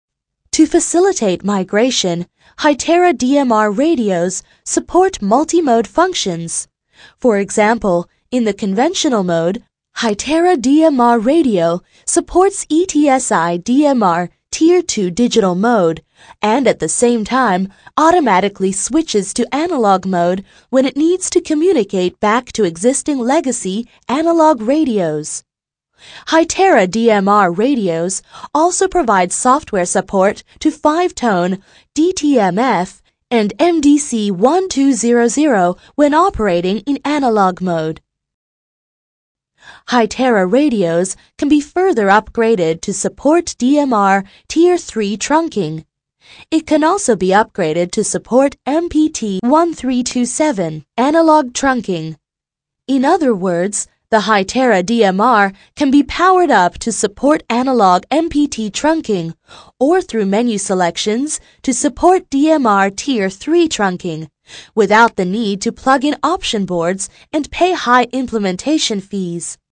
Hytera DMR Multi Mode - tryb cyfrowy i analogowy
Urządzenia Hytera DMR pracują w oparciu o otwarty standard ETSI DMR i technologię TDMA. Urządzenia  Hytera DMR  mogą  pracować  w  konwencjonalnym trybie analogowym lub cyfrowym. Pozwala to na migrację z systemu analogowego na cyfrowy, radiotelefon po radiotelefonie, kanał po kanale lub system po systemie. Wszystkie radiotelefony Hytera DMR obsługują także  rozszerzoną sygnalizacją 5-tonową w standardzie.